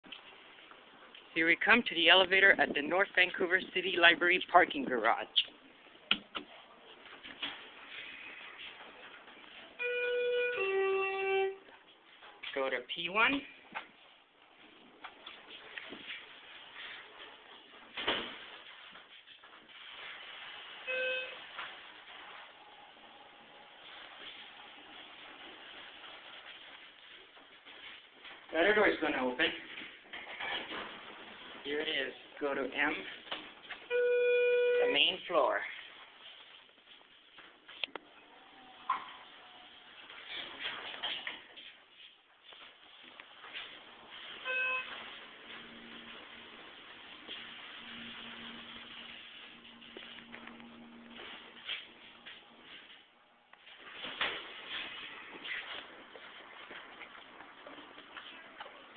Schindler Elevator at North Vancouver City Library Car No. 3
Riding the elevator at North Vancouver City Library parking garage. The building offically opened yesterday.